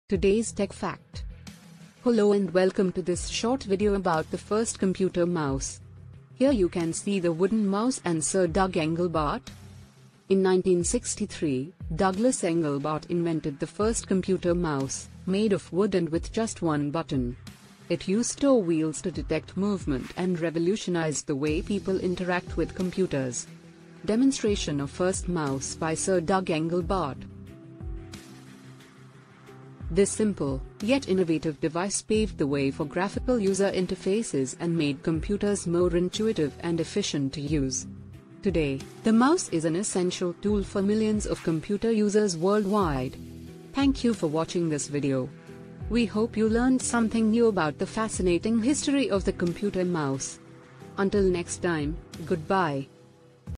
All About Invention of Mouse !!
The computer mouse was invented by Douglas Engelbart in 1963, while he was working at the Stanford Research Institute. The mouse was originally created as a way to interact with a computer in a more intuitive and user-friendly way, allowing users to point, click, and scroll on a graphical user interface (GUI). Engelbart's design consisted of a wooden shell, with two metal wheels and a ball on the bottom that could detect movement in two dimensions. This design was later refined and miniaturized by companies such as Apple and Microsoft, leading to the compact, plastic mice that are commonly used today.